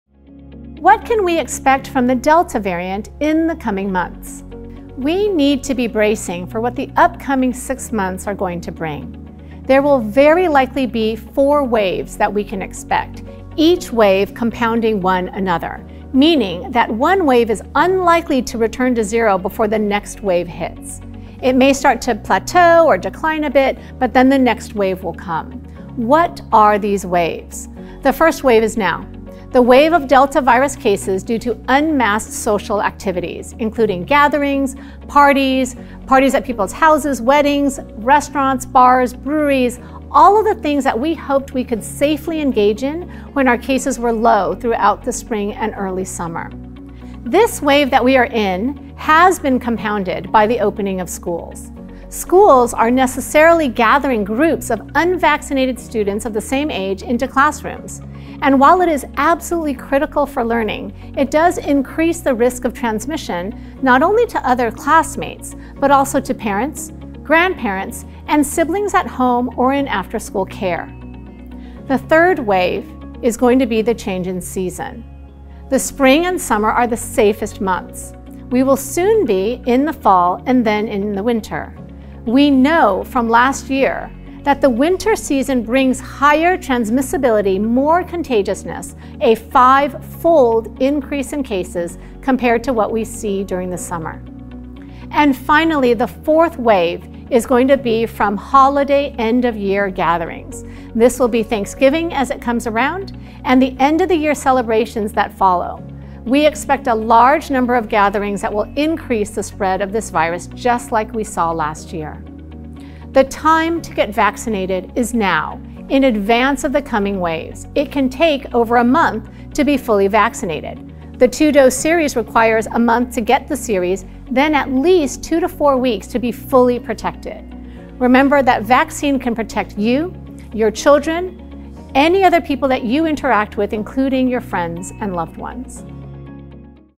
What can we expect from the delta variant in the coming months?
Dr. Susan Huang, medical director of epidemiology and infection prevention at UCI Health, details the four waves of the COVID-19 delta variant we can expect over the next six months, and explains why now is the time to get vaccinated.

Get patient stories, wellness tips, health care news and more on our Live Well blog